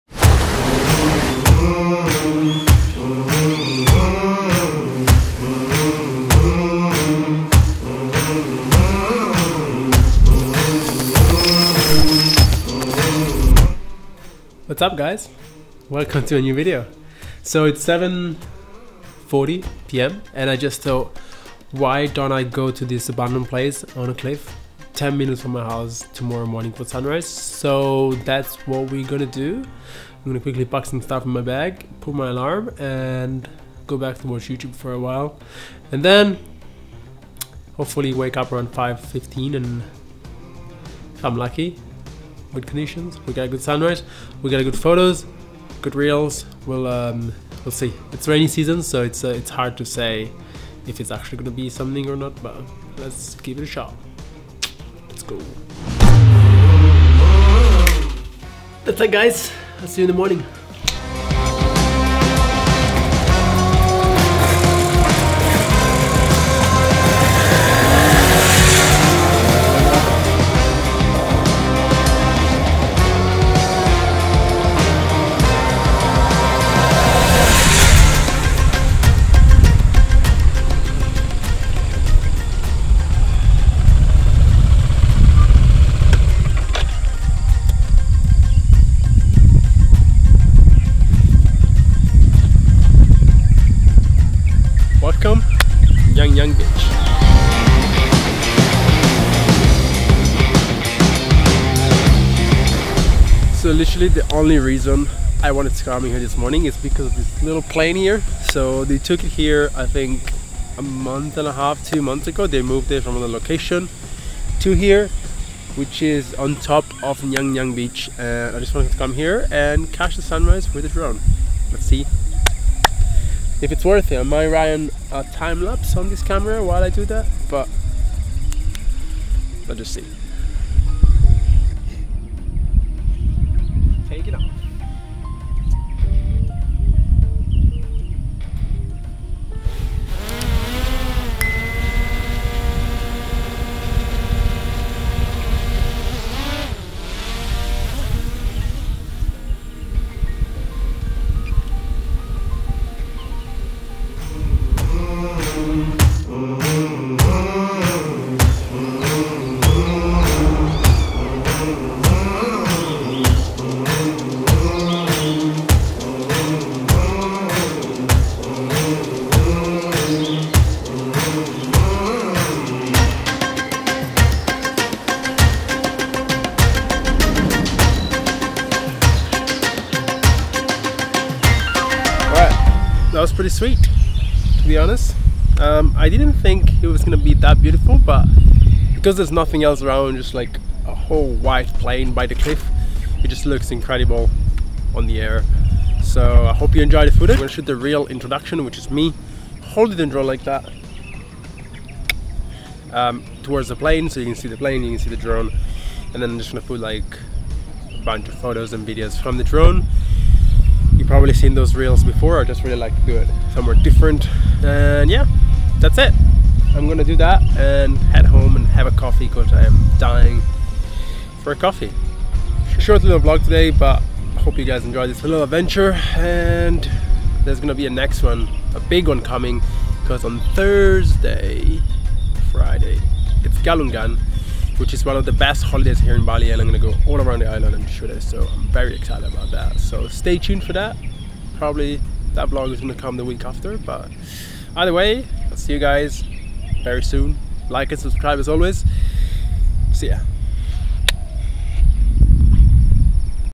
Found An ABANDONED Airplane?!
Little photography vlog to go chase the sunrise just a few minutes from my house in Uluwatu.